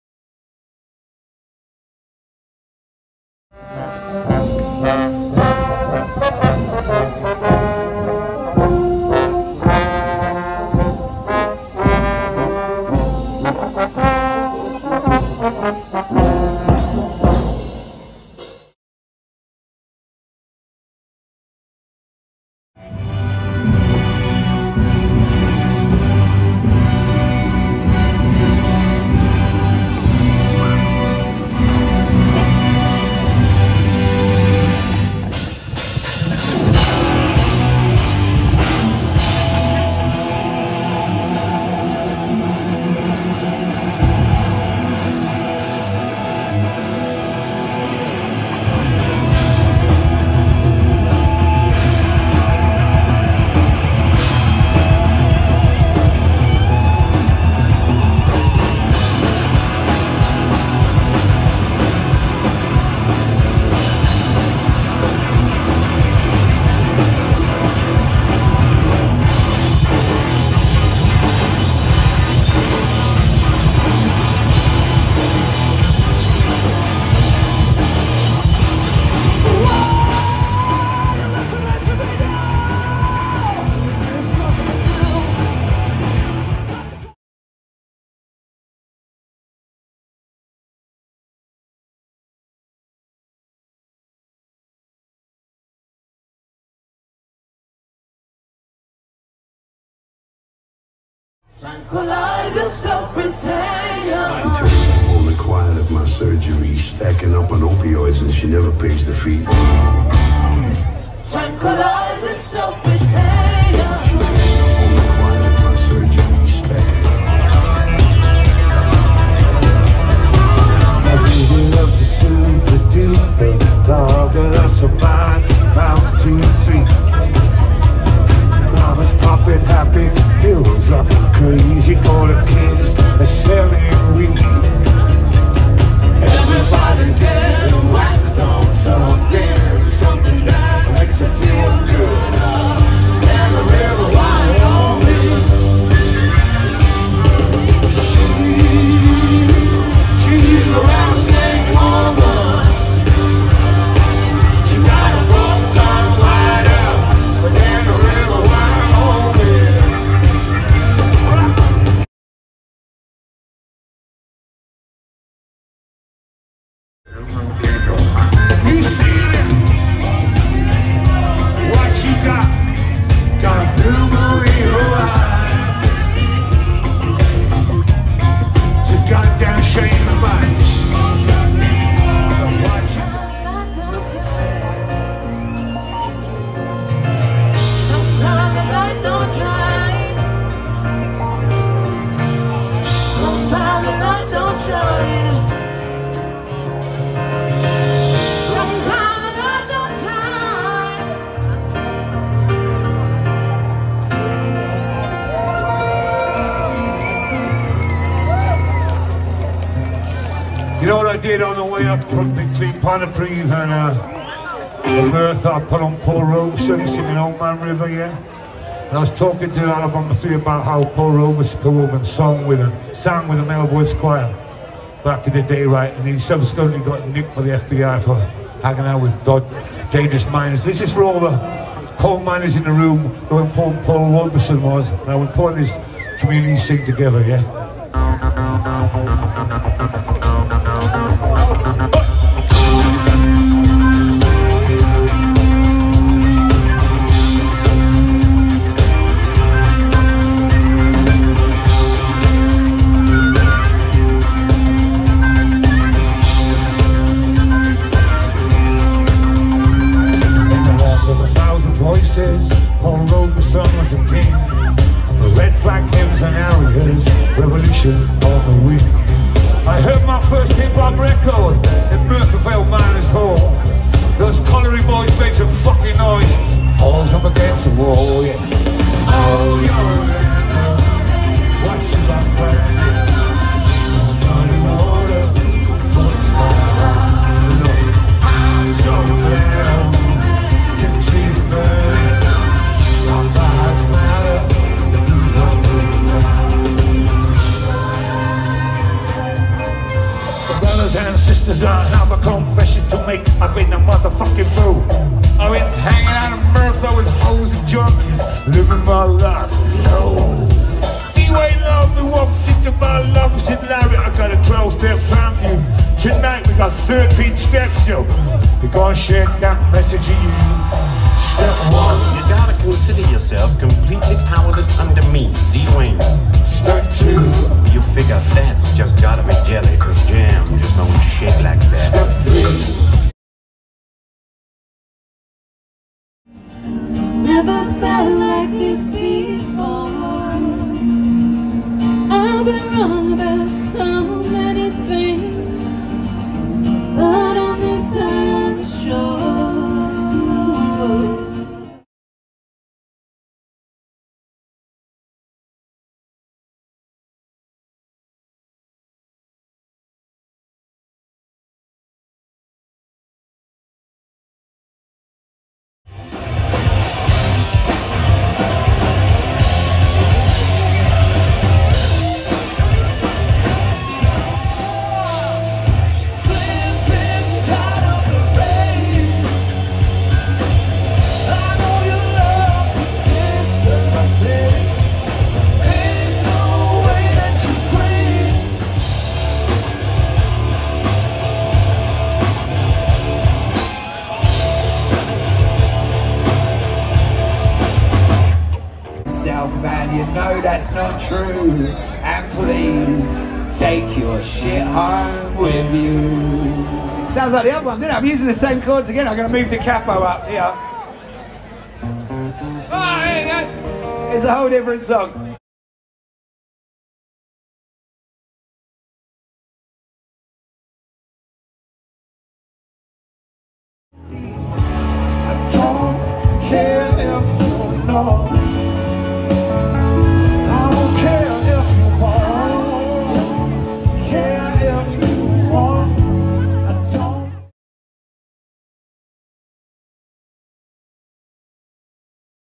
Merthyr Rising 2022
I checked my phone and found random videos and photos to stick in a video. It was great to have the Merthyr Rising festival back in 2022.